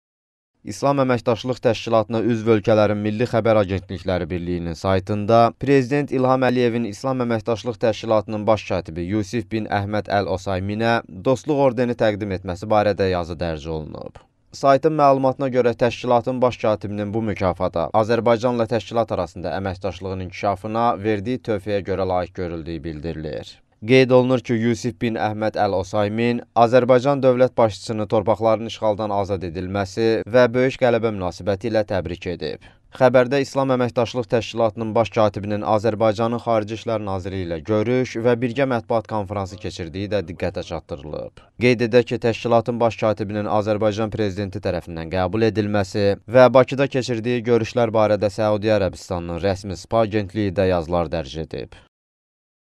"Dostluq" ordeni İƏT mətbuatında
Təşkilatın saytında İ.Əliyevin İƏT-in baş katibinə təqdim etdiyi orden diqqət çəkib